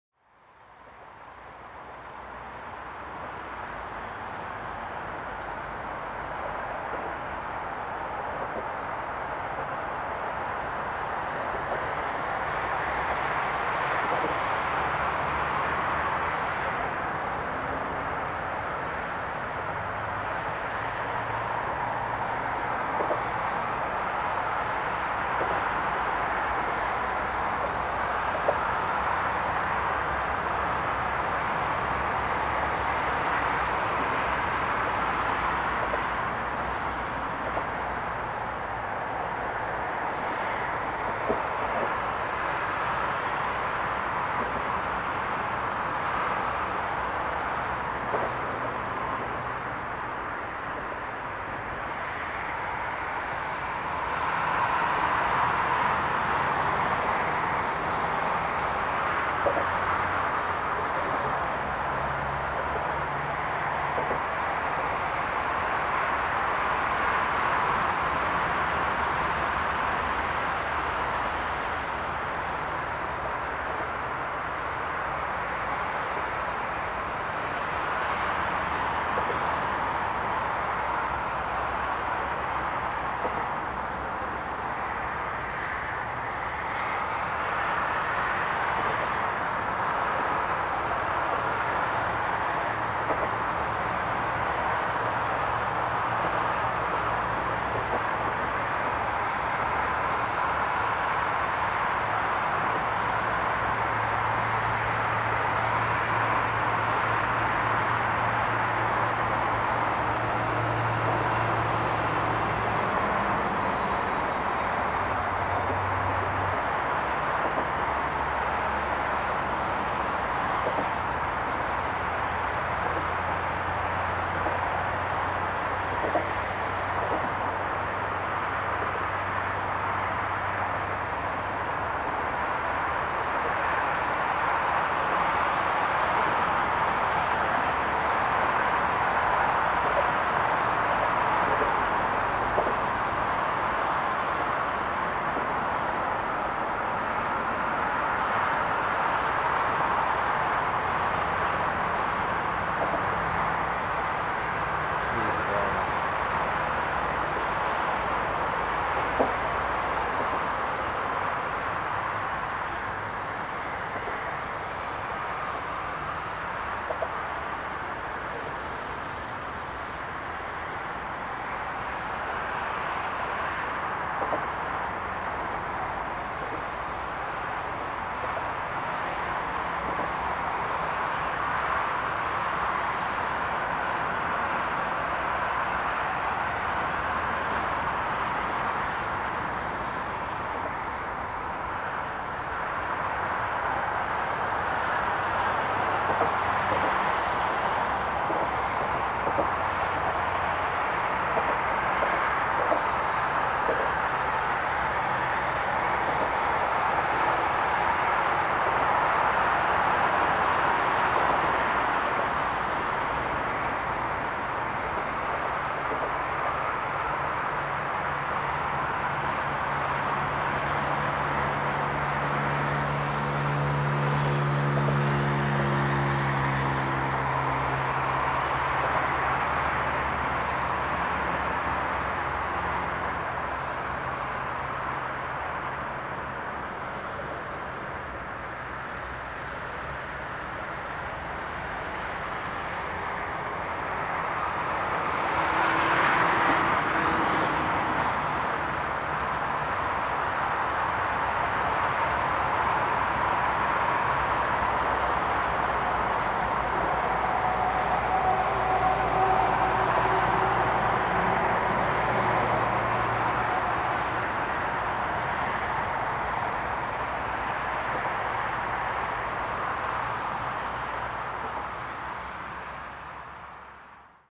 Birds above Brno
Birds forming into shapes above Brno.